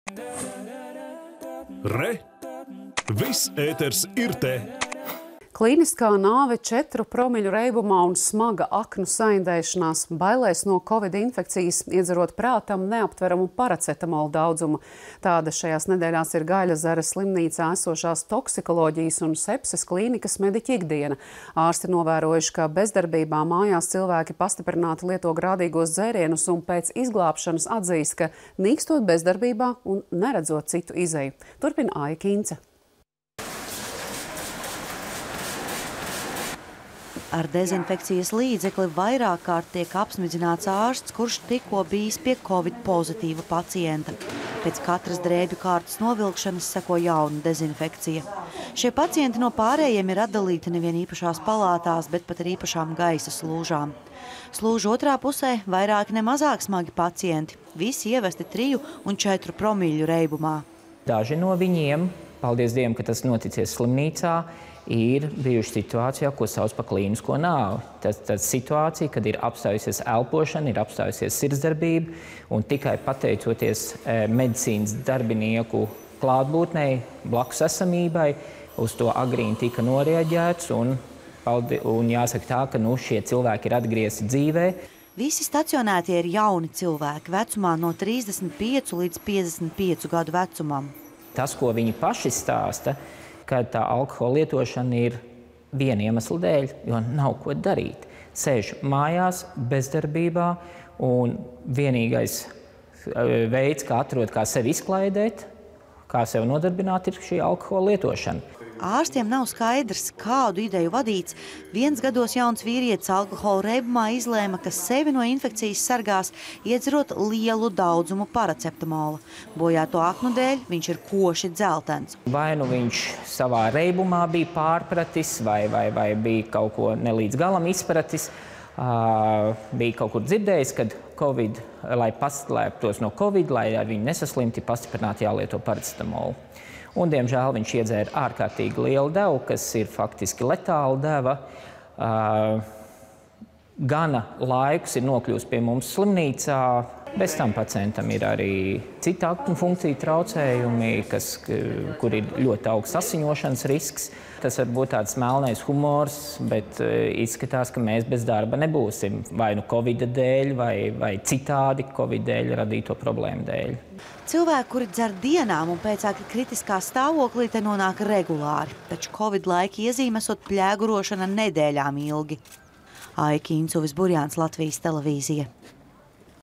Covid-19 krīzē pieaug alkoholisms
Klīniskā nāve četru promiļu reibumā un smaga aknu saindēšanās, bailēs no Covid infekcijas iedzerot prātam neaptveramu paracetamola daudzumu. Tāda šajās nedēļās ir “Gaiļezera” slimnīcā esošās Toksikoloģijas un sepses klīnikas mediķu ikdiena. Ārsti novērojuši, ka bezdarbībā mājās cilvēki pastiprināti lieto grādīgos dzērienus un pēc izglābšanas atzīst, ka nīkstot bezdarbībā un neredzot citu izeju. Turpina Aija Kinca